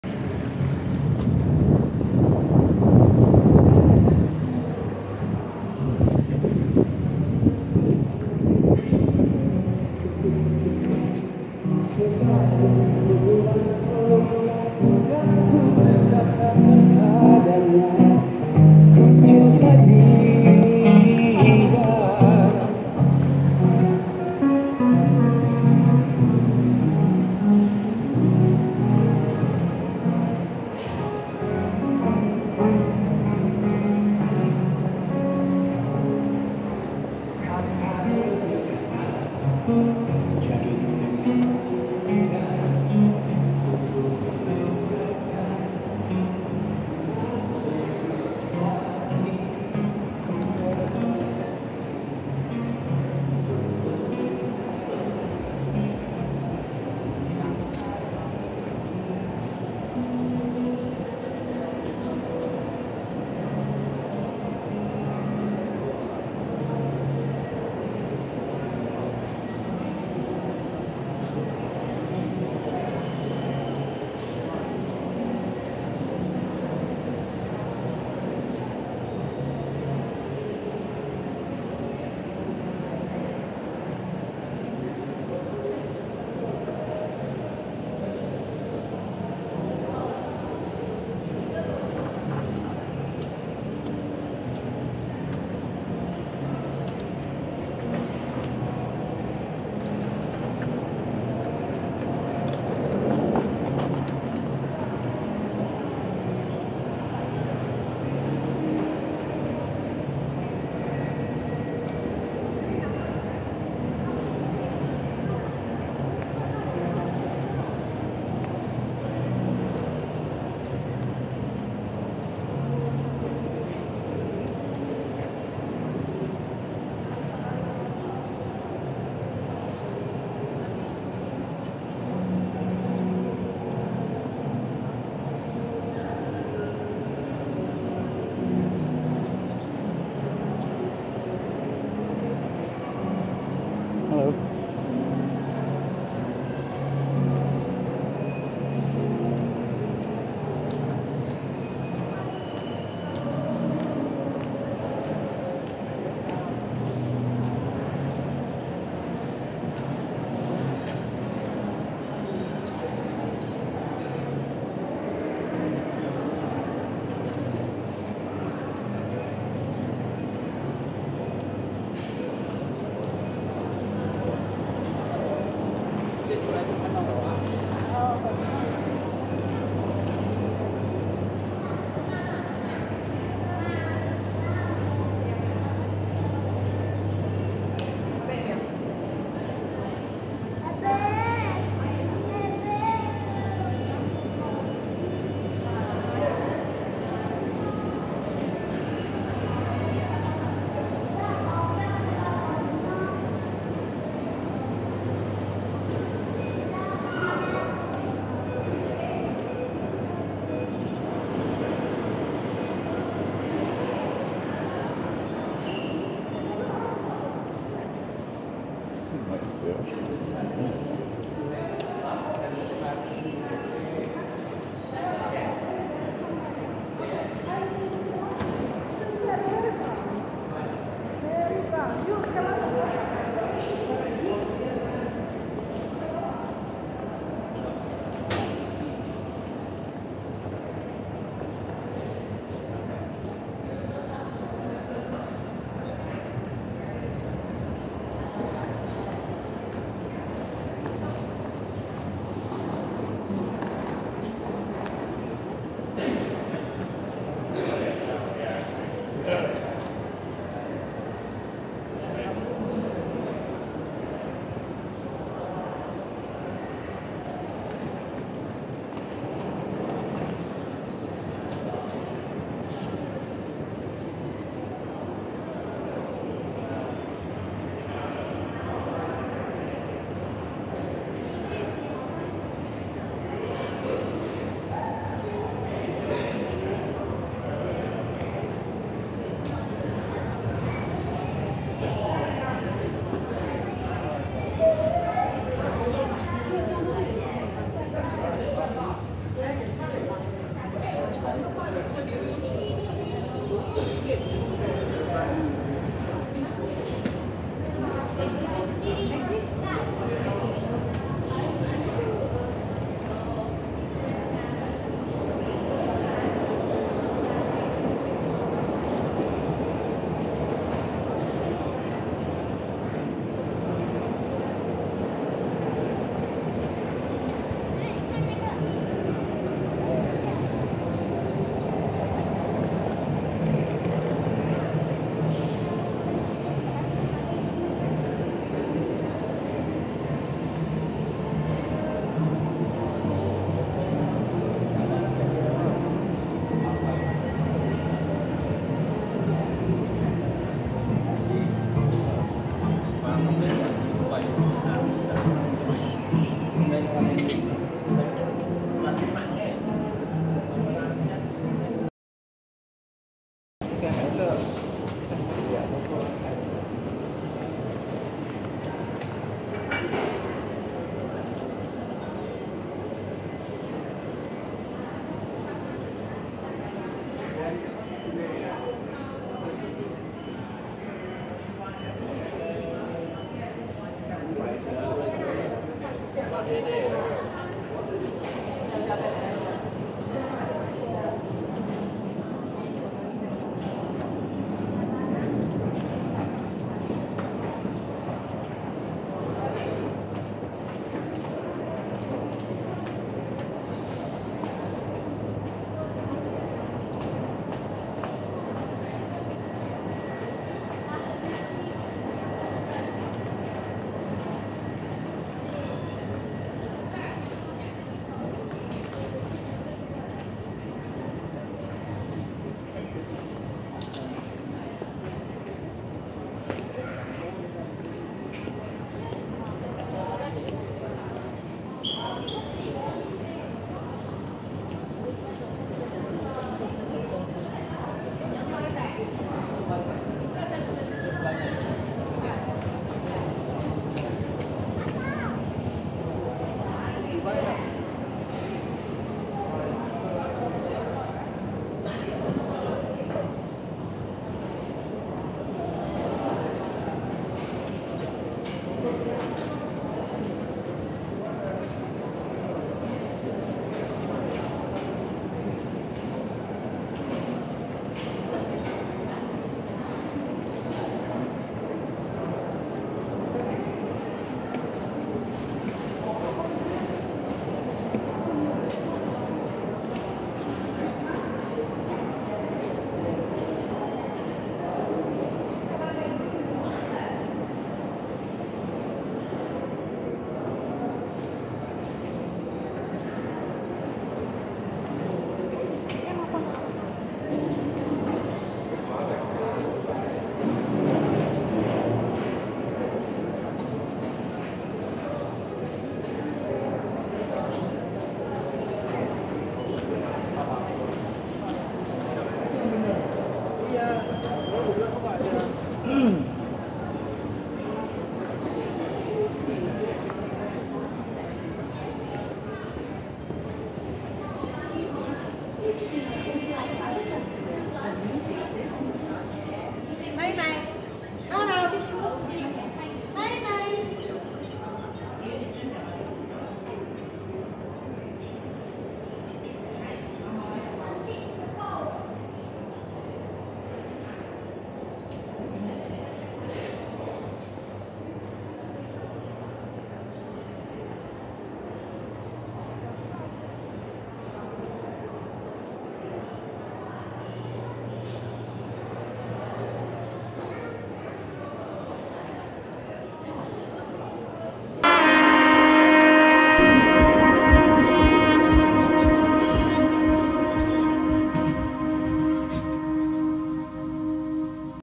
Senai Airport, Johor 🇲🇾
Senai airport is a regional airport based in Johor, Malaysia
Come with me and have a look around.

I walk around and show you places without talking.

If you wish to help with a contribution to my vlog or buy me a kopi you can